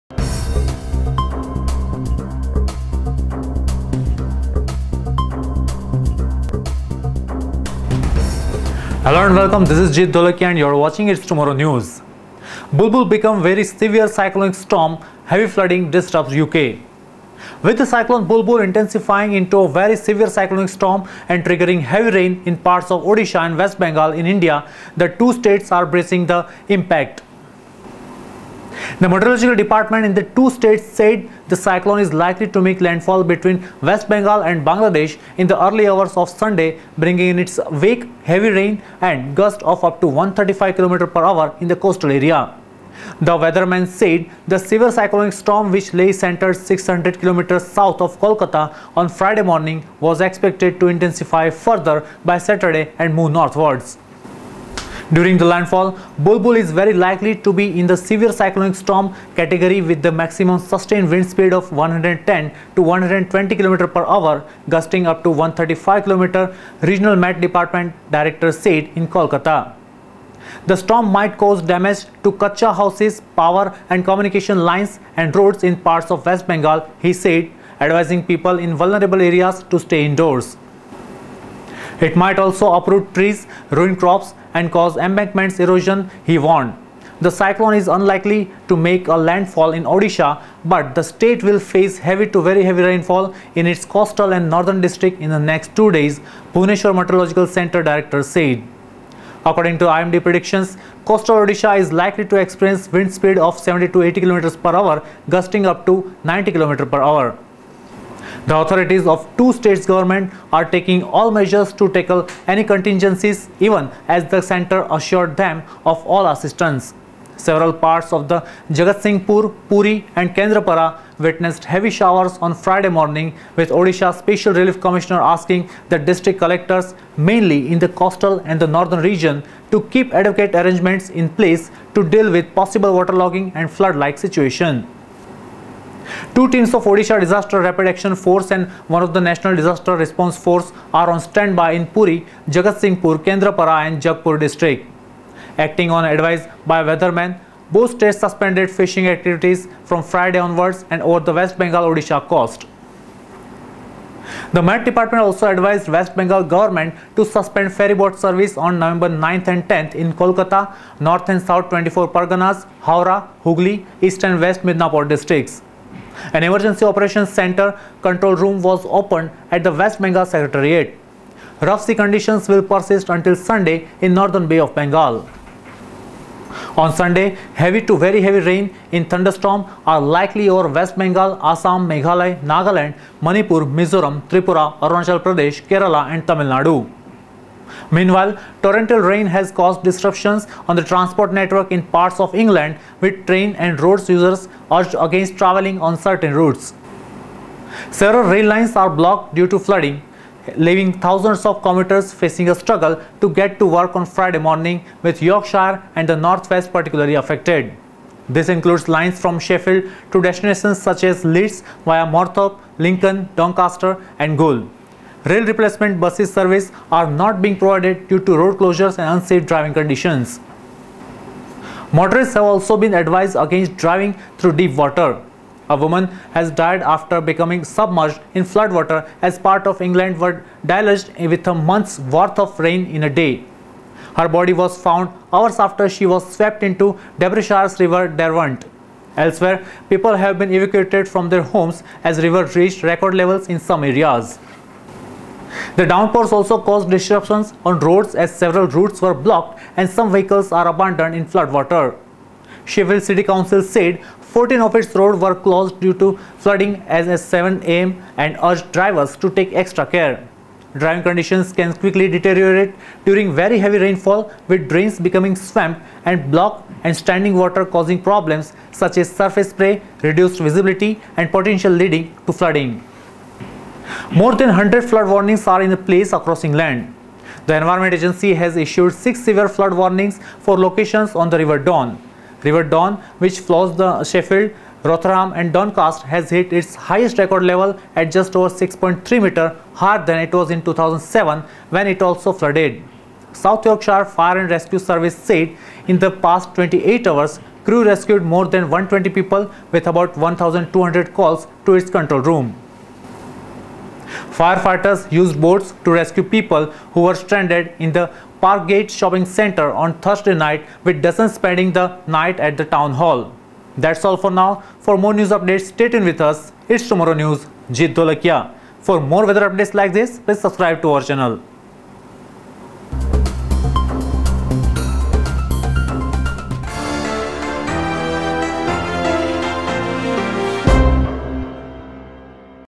Bulbul becomes very severe cyclonic storm, Heavy flooding disrupts UK
With the cyclone 'Bulbul' intensifying into a very severe cyclonic storm and triggering heavy rain in parts of Odisha and West Bengal in India, the two states are bracing for the impact. The Met department in the two states said the cyclone is likely to make landfall between West Bengal and Bangladesh in the early hours of Sunday, bringing in its wake heavy rain and gusts of up to 135 kmph in the coastal areas.

Meanwhile, torrential rain has caused disruption on the transport network in parts of England, with train and road users urged against travelling on certain routes. Several rail lines are blocked due to flooding, leaving thousands of commuters facing a struggle to get to work on Friday morning, with Yorkshire and the North West particularly affected.